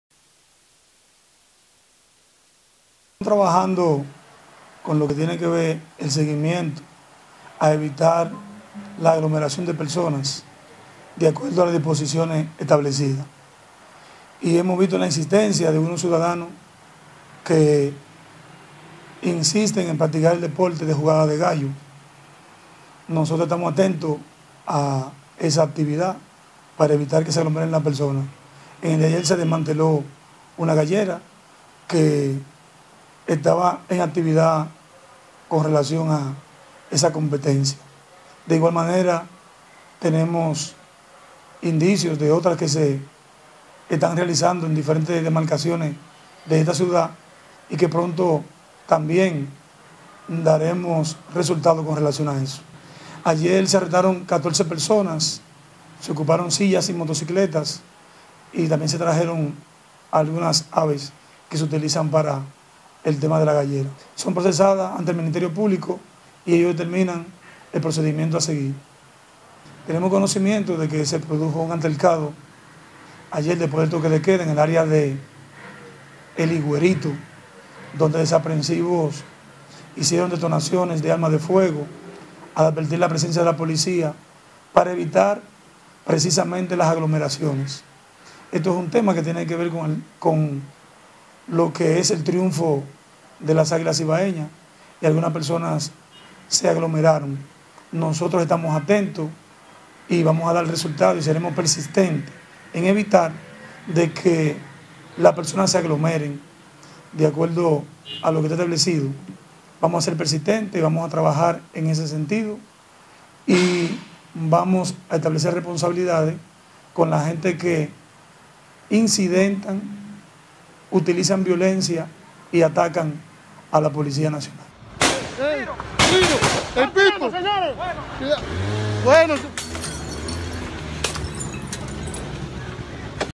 Coronel dice fueron apresado 14 personas, incautaron silla, motocicletas y decenas gallos en gallera
Coronel de Moca Bell Fernández nos informa fue desmantelada una gallera en San Víctor a Riba dónde fueron apresada 14 personas, incautaron silla, motocicletas y decenas de gallos.